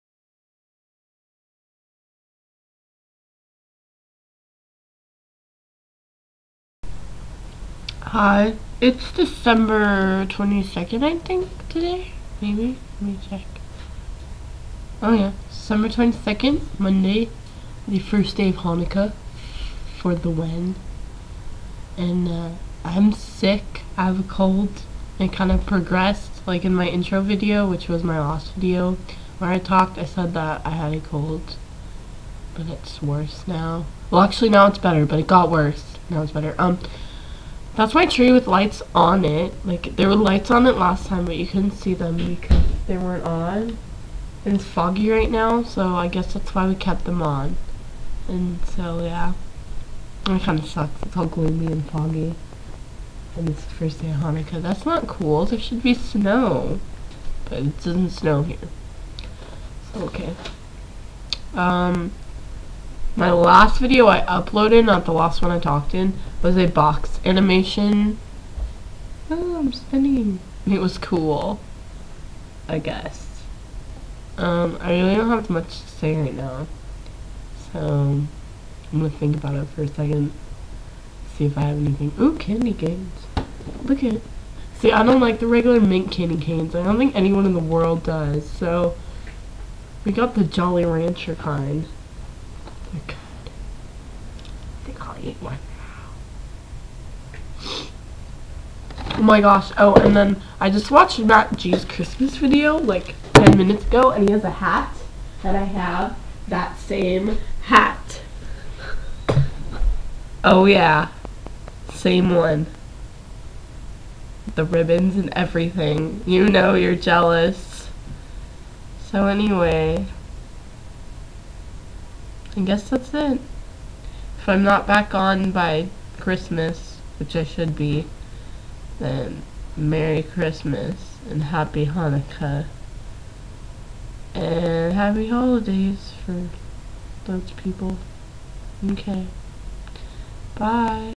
Vlog 1- Thalia's Blog
My first vlog. I have a nasty cold so I dont sound to great and you will notice everyone in my house is in bathrobes. lol.